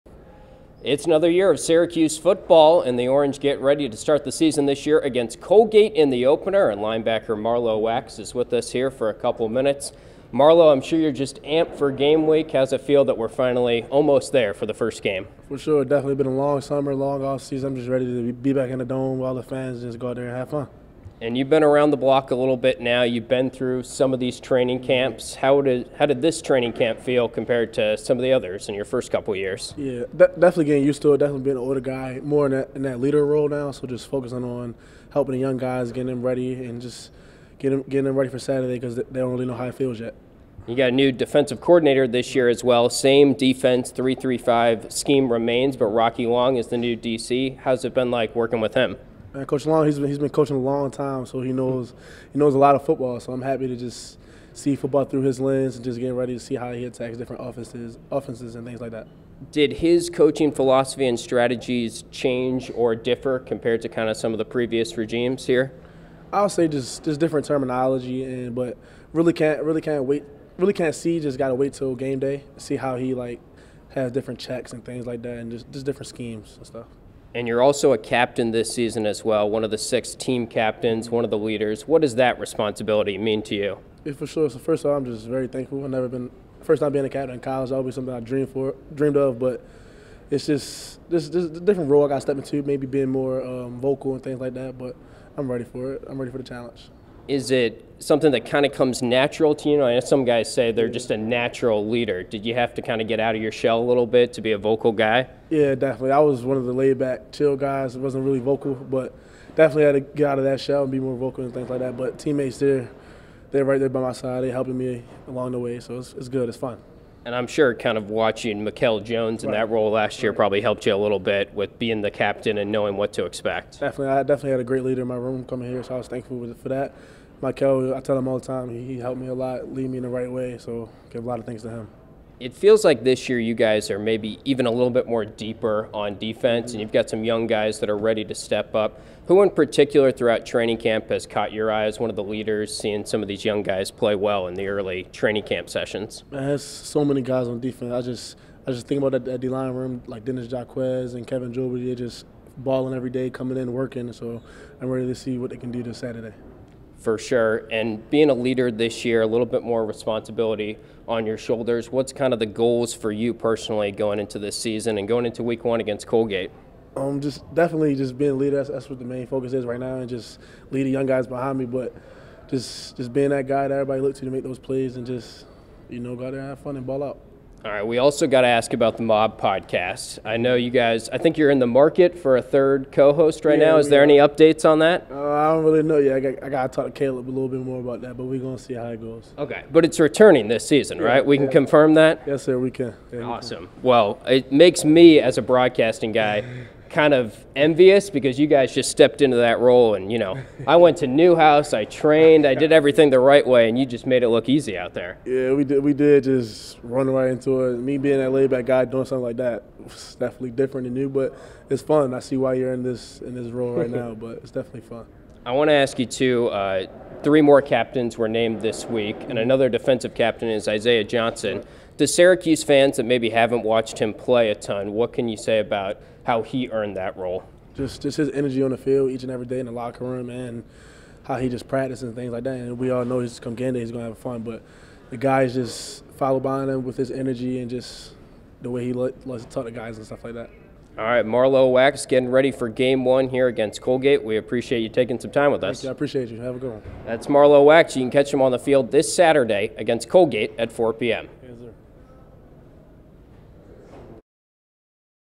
Marlowe Wax Interview
Linebacker Marlowe Wax previews Saturday's season opener vs. Colgate.